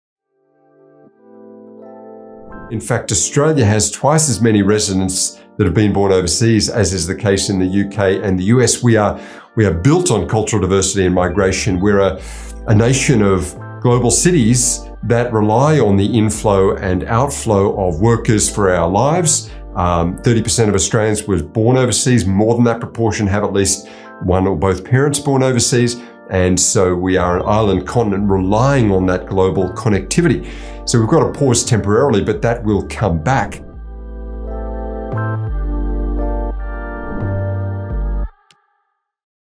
Australia's cultural diversity - Australia towards 2031
Insights from the latest report, Australia towards 2031, freely available to download

About McCrindle

Get in touch:

Located: Sydney, NSW